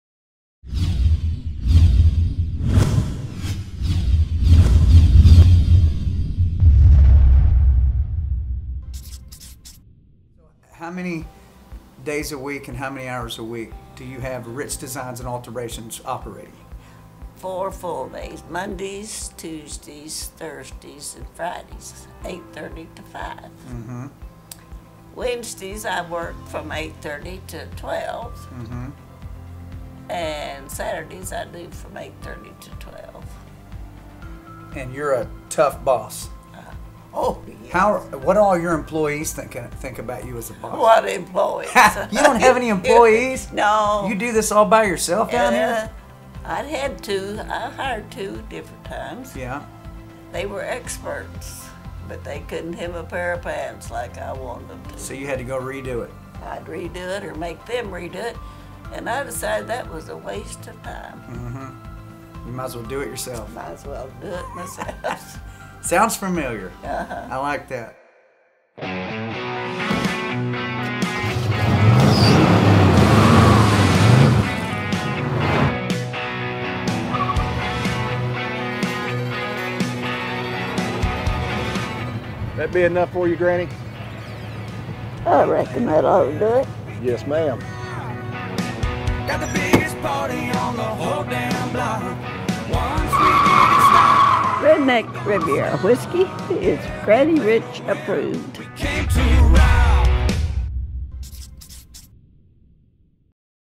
Granny Rich Reserve!
ohn Rich talks to his Granny Rich about her amazing work ethic and how see likes to make sure the job gets done right!